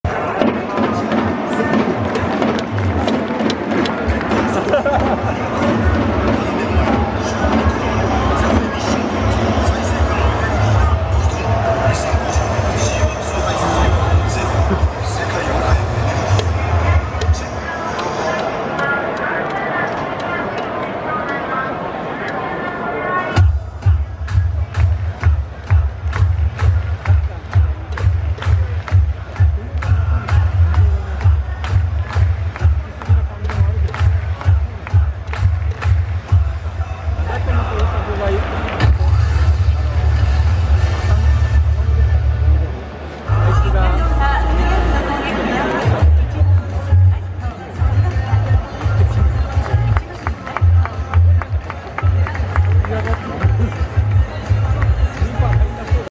佐藤輝明と後藤コーチ 雑談して仲がいい(笑) 2021-10-14
相手チームのコーチと何を話しているのでしょうか？世間話でしょうか。仲いいですね。

阪神藤浪がヤクルト宮本にデッドボール｡宮本が落ち着いてから､宮本を見てちゃんと謝る藤浪｡